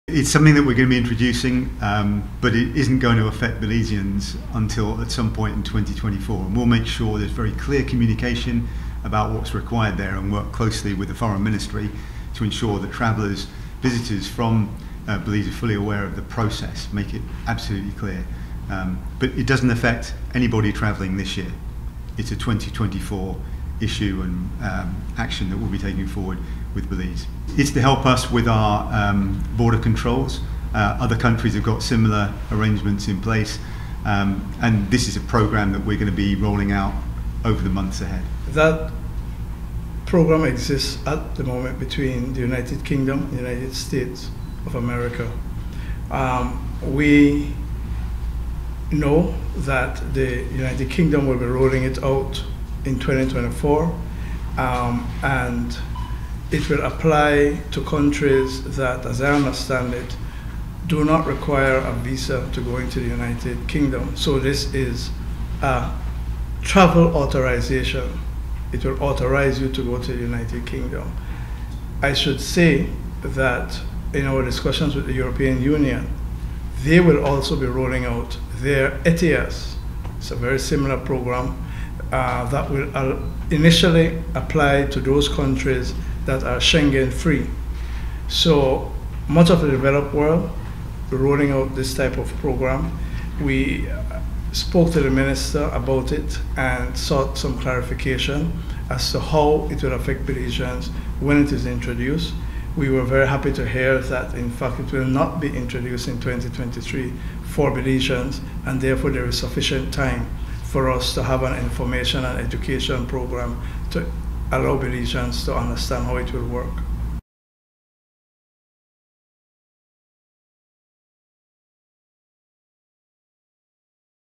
UK Introduces Electronic Travel Authorization (ETA) Requirement
Next year, Belizeans traveling to the United Kingdom will need an Electronic Travel Authorization (ETA) to enter the country. The UK is moving to increase its border security and believes the measure will aid in irregular migration. The document will be electronically linked to your passport after the measure goes into effect. Yesterday, the UK’s Minister for the Americas and the Caribbean, David Rutley, and Belize’s Foreign Minister, Eamon Courtenay, weighed in on the decision and explained that an exact enforcement date has not been decided on.